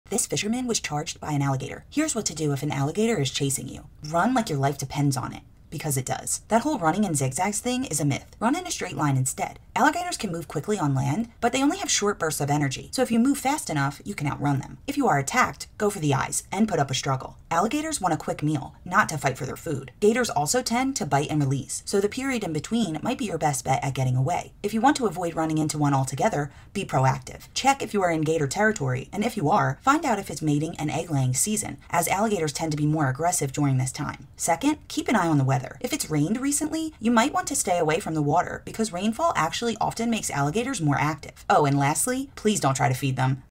What to Do If an Alligator Chases You
This fisherman was caught off guard by a charging alligator. Here's what you should do if you're ever in this scary situation. Hint: Run fast. 🐊